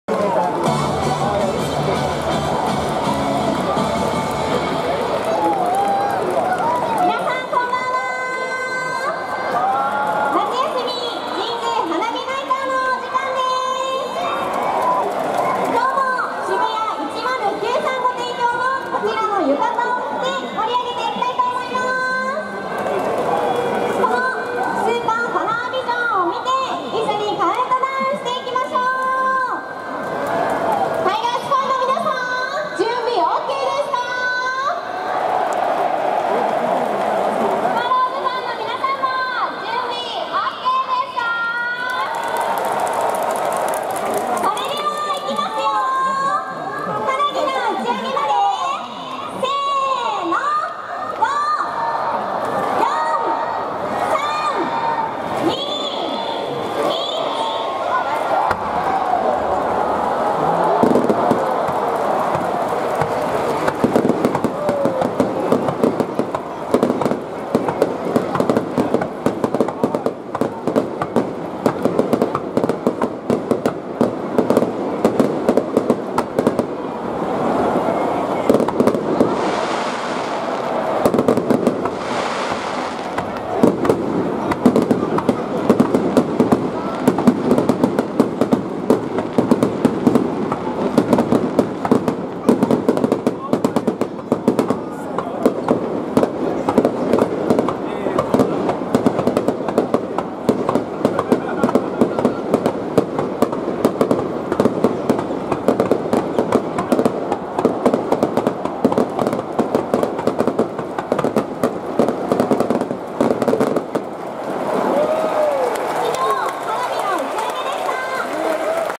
20160807 夏休み！神宮花火ナイター 花火300発！！
20160807 神宮球場恒例　夏の花火です。